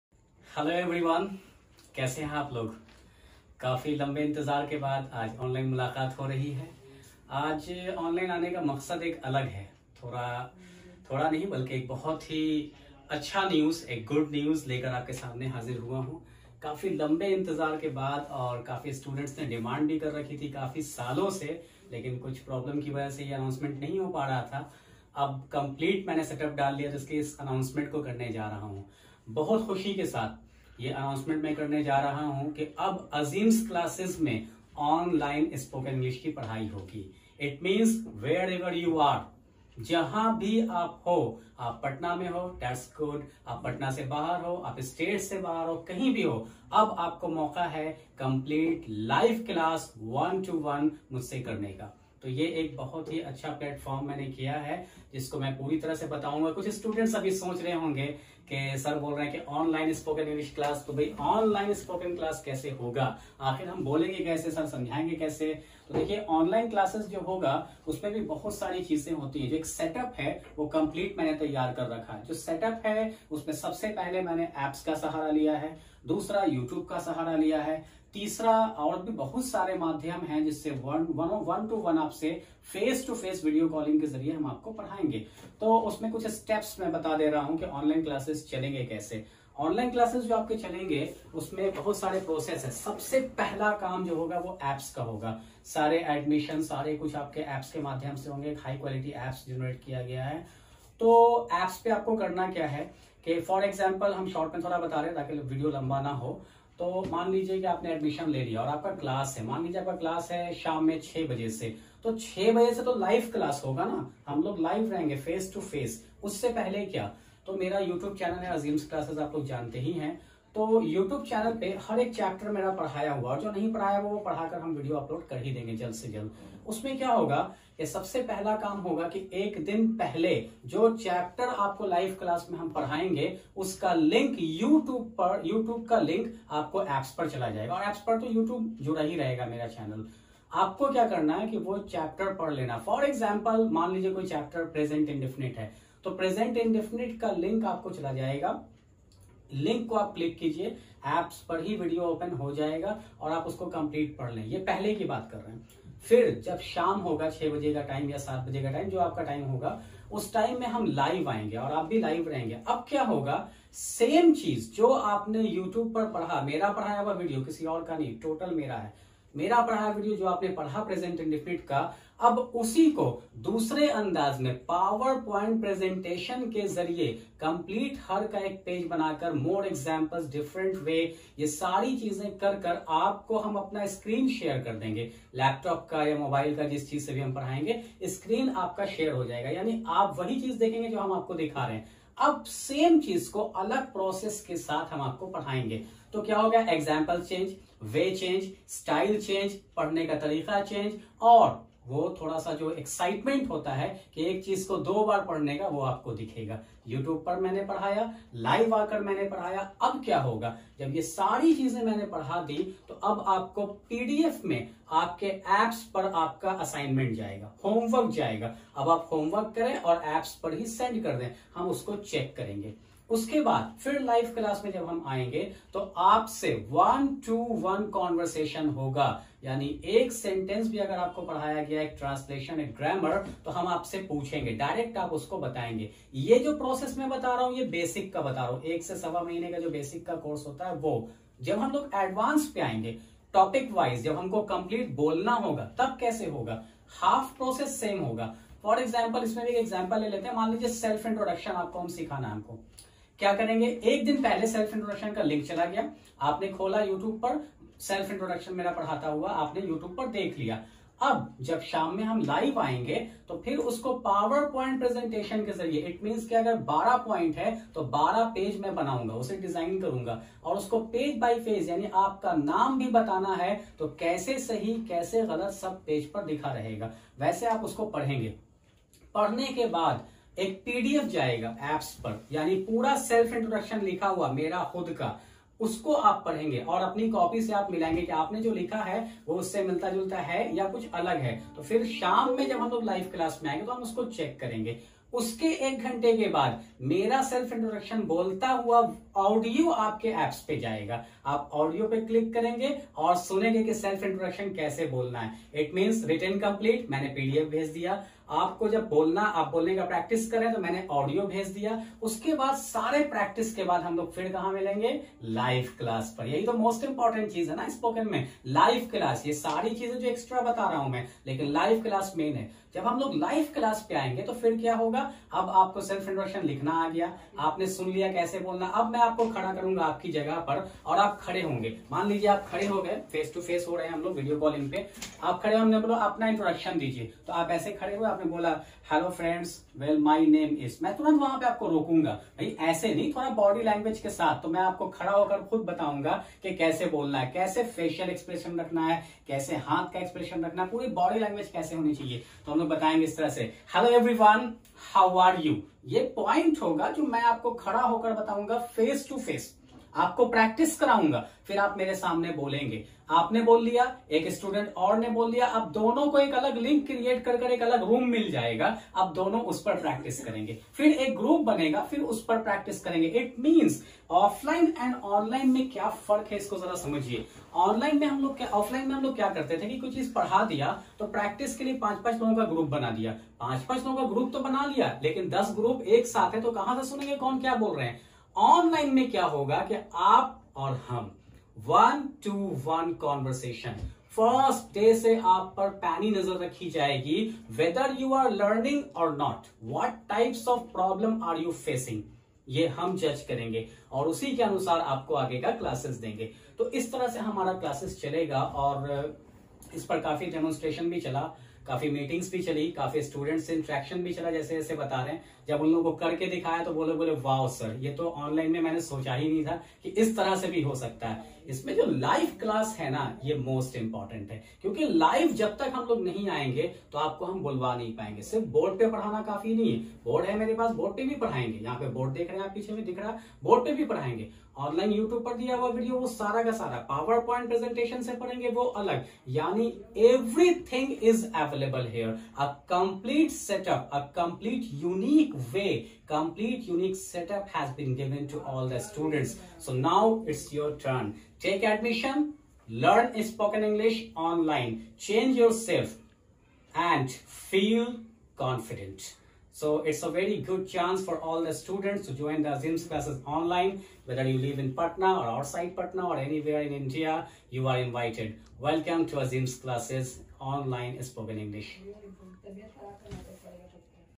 Learn Spoken English online | with Azeem's classes
We are so excited to inform that Azeem's Classes is now going to start Online ecosystem For the students to Learn Spoke English from there home with a premium experience.

We will not only provide pre recorded Video lectures but online classes will also be given in order to make the course more interactive.

For the Oral and speech practices Azeem sir will take personal online classes for improving the speaking skills of every individual student.

the Live classes will be different from the pre recorded Videos.